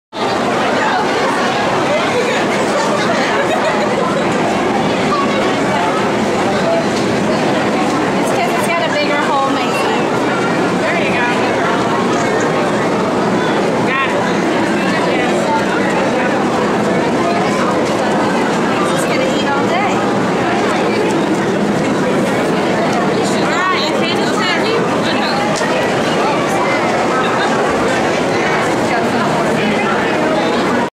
Maisy and Madi Feeding a Giraffe
At the 2010 State Fair of Texas, Maisy and Madi try to feed a giraffe, but he's not interested in their tiny cups of feed and instead goes for the larger cup.

(Technical note: This is raw, only slightly edited footage shot using my new HD camcorder. I'm importing it as a test of the camera's video quality and how well HD video looks when pushed to YouTube.)